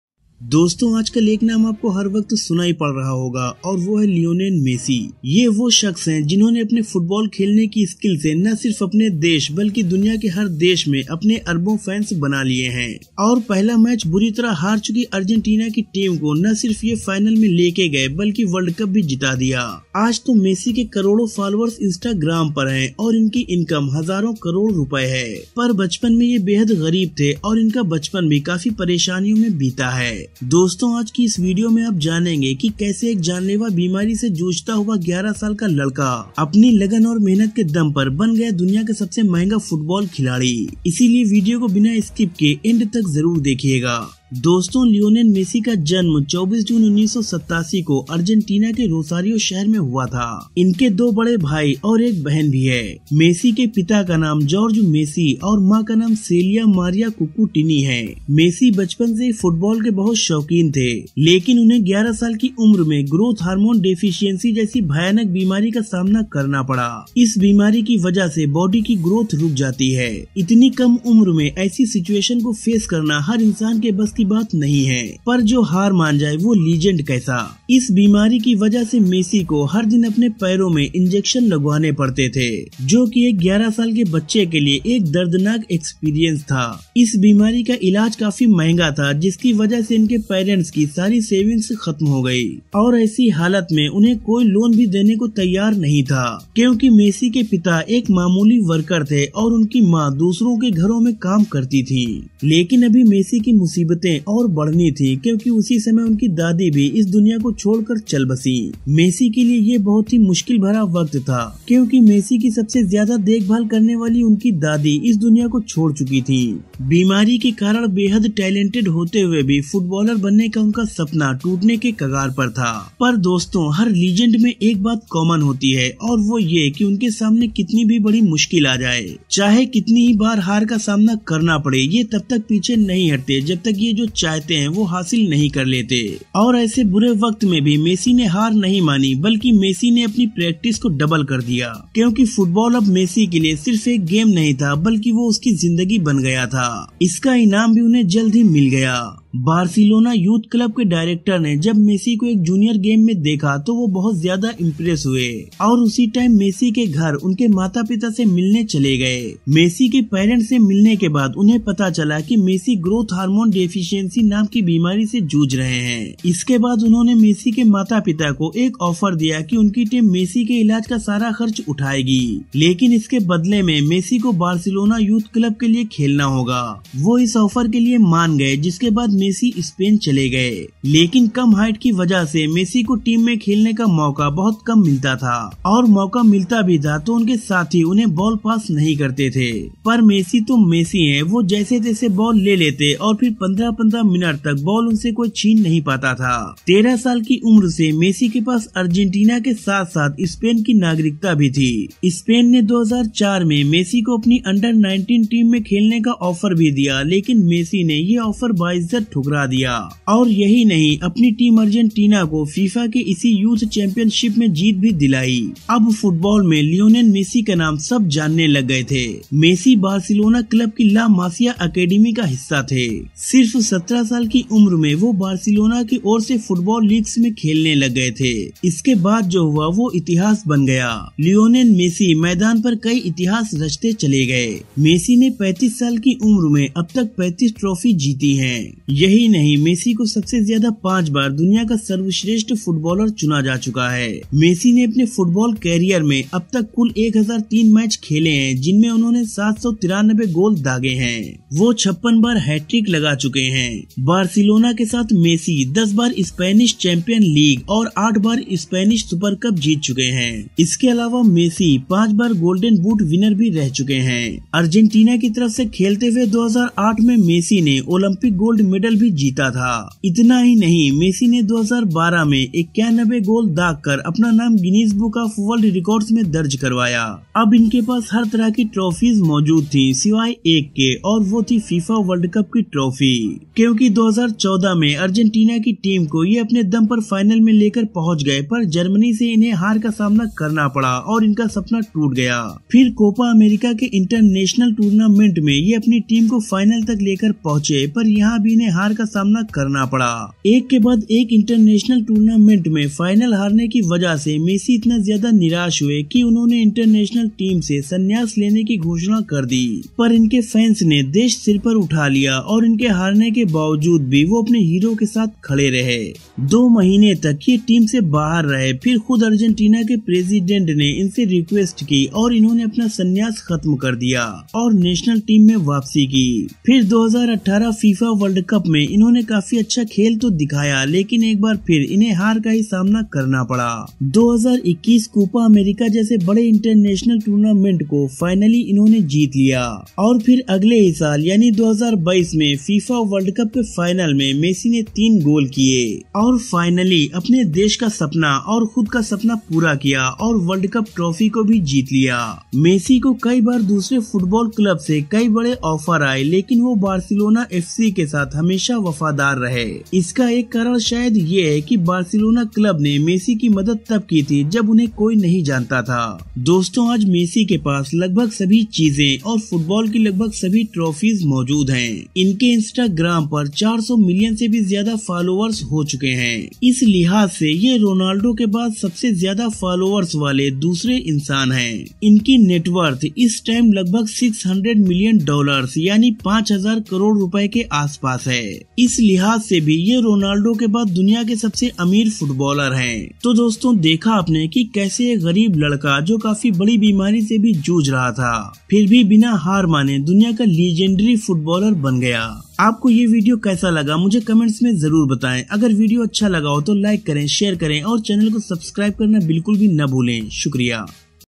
मज़दूर का बेटा कैसे बना दुनिया का सबसे अमीर खिलाड़ी? The Incredible Story of Lionel Messi
Hello Friends, in this video we will be talking about Lionel Messi मज़दूर का लड़का कैसे बना सबसे अमीर Footballer? Success Story of Messi. I hope you will like this video.

Thanks